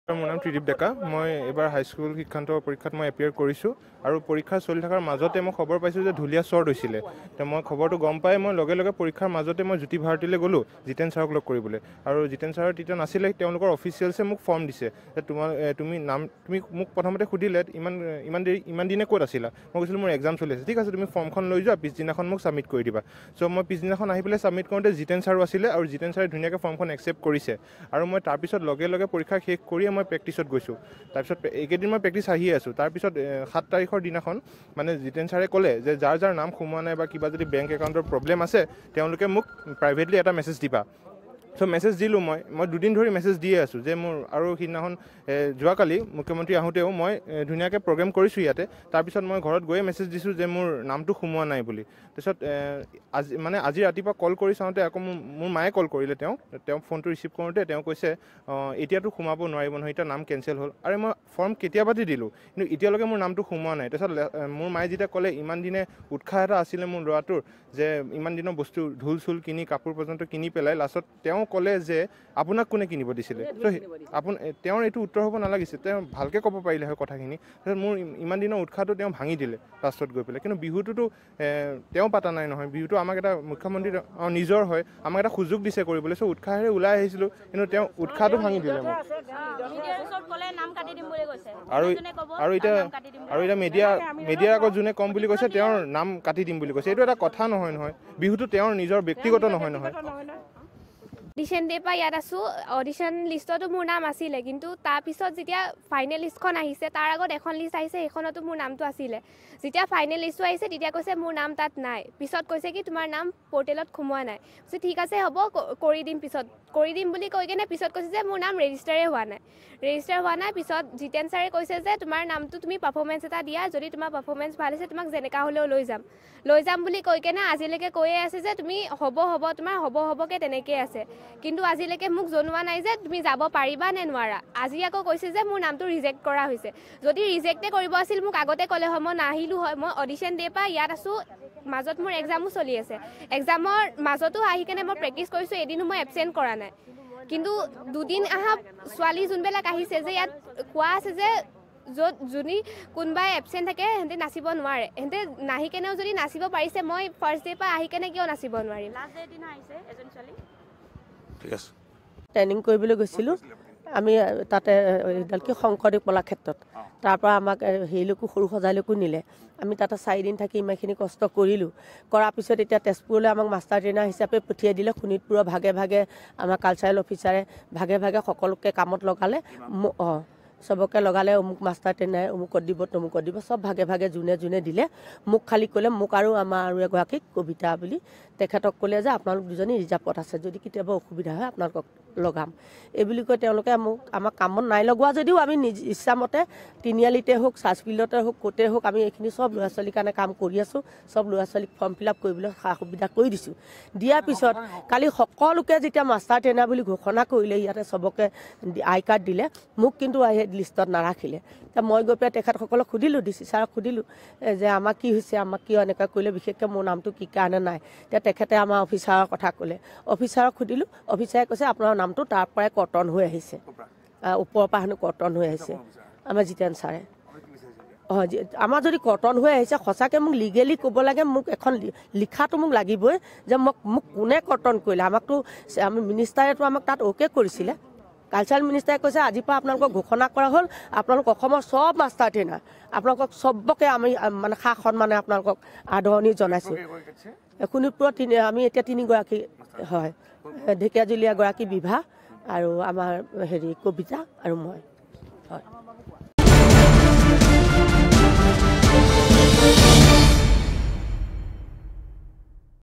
তেজপুৰত বিহুৱা বিহুৱতীক লৈ স্বজন-তোষণ ৰাজনীতি!
📌ধনৰ লেনদেনৰ জৰিয়তে বিহুৱা বিহুৱতী বাচনিৰ গুৰুতৰ অভিযোগ বঞ্চিত বিহুৱা বিহুৱতীৰ
📌শুনক ভুক্তভোগীৰ মুখেৰে